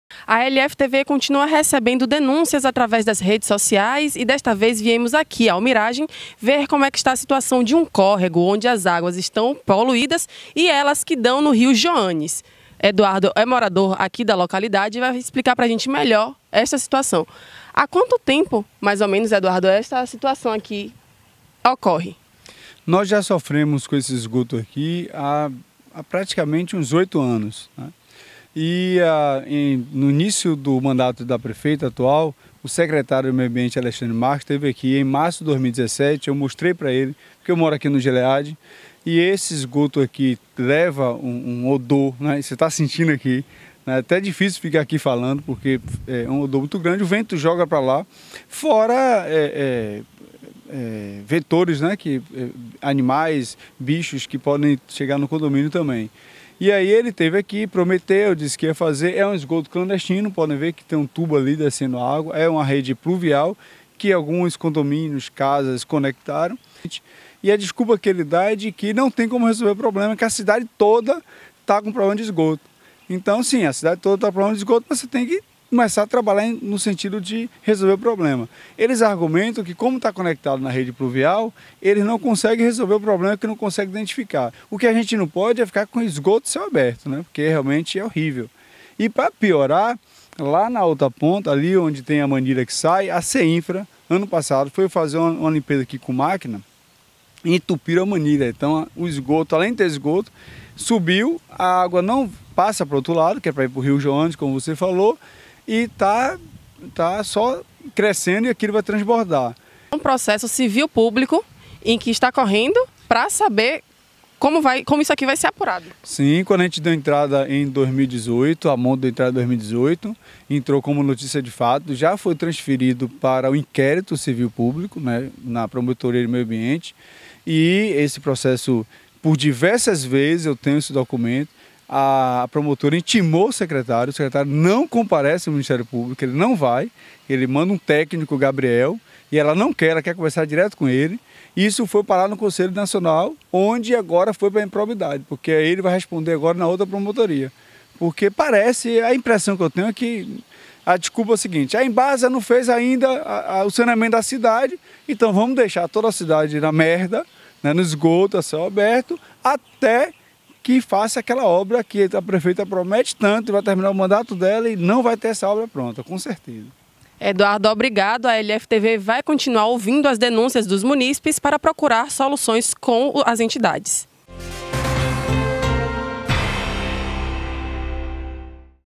Mau cheiro do esgoto a céu aberto no bairro de Miragem
Moradores do Miragem sofrem com o mau cheiro do esgoto a céu aberto na localidade e denunciam que mesmo solicitando, a prefeitura não resolve o problema.